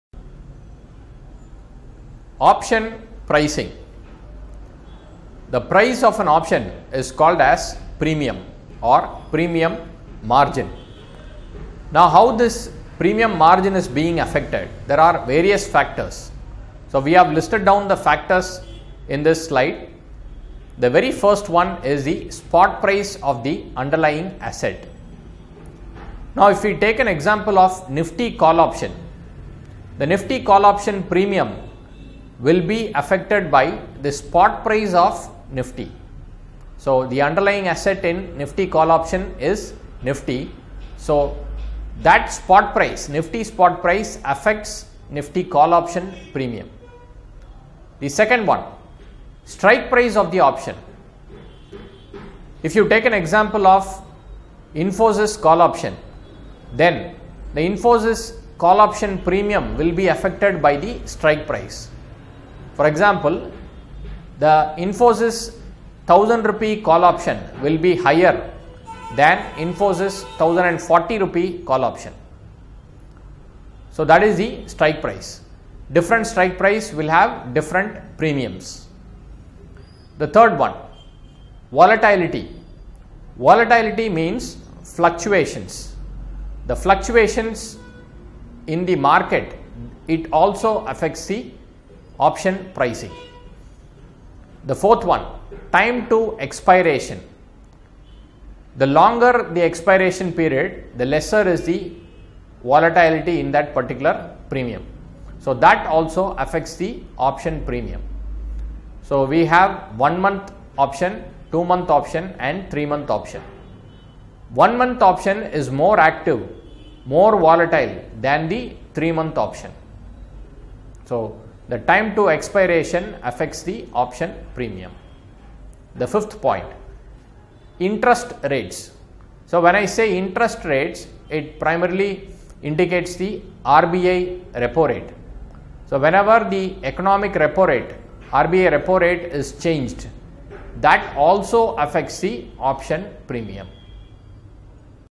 Option Pricing | Factors Affecting Option Premium | Equity Derivatives
Option Premium ( price of an option ) is affected by various factors.
Option pricing
1) Spot price of the underlying asset
2) Strike price of the option
3) Volatility of the underlying asset’s price
4) Time to expiration
5) Interest rates